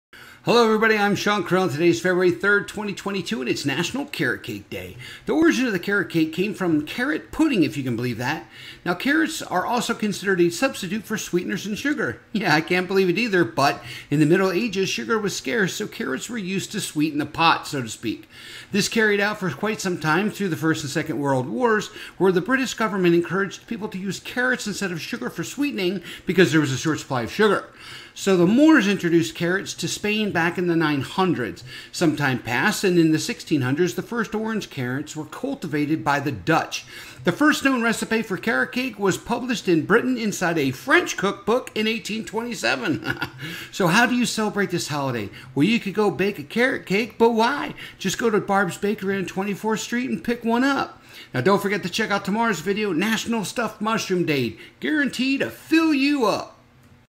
February 3, 2022 - National Carrot Cake Day
Hello everybody, I’m Shawn Krill and today is February 3rd, 2022 and it’s also National Carrot Cake Day.

The origin of carrot cake came from carrot pudding if you can believe that.  Carrots are also considered a substitute for sweeteners and sugar.  Yea I can’t believe it either but in the Middle Ages sugar was scarce so carrots were used to sweeten the pot so to speak.  This carried out for quite some time through the 1st and 2nd world wars where the British government encouraged people to use Carrots instead of sugar for sweetening because there was a short supply of sugar.

So, the moors introduced the carrots to Spain back in the 900’s.  some time passed and in the 1600’s the first orange carrots were cultivated by the Dutch.  The first known recipe for Carrot Cake was published in Brittan inside a French cookbook in 1827

So how do you celebrate this holiday, well you could bake a carrot cake but why? just go to Barb’s Bakery on 24th street and just pick one up.

Don’t forget to check out tomorrow’s video. “National Stuffed Mushroom Day”. Guaranteed to fill you up.